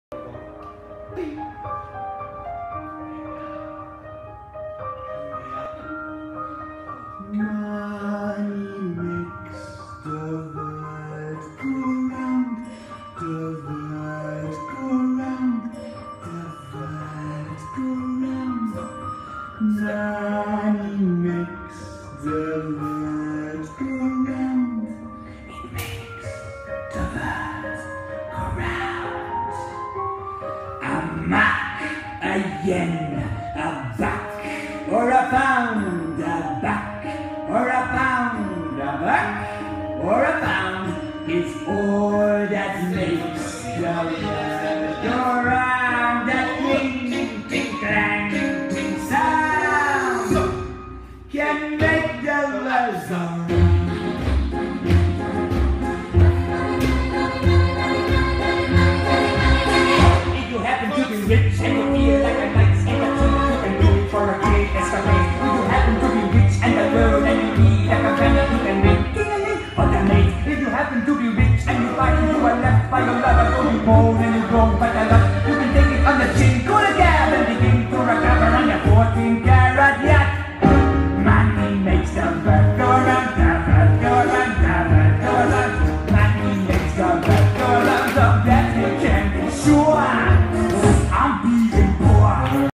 Rydbergs Dance Academy
"Prova på" dag på Rydbergs
Musikaljazz för Magnus Lundgren
Sista lektionen och alla elever har provat på 2-3 klasser efter varandra utan paus.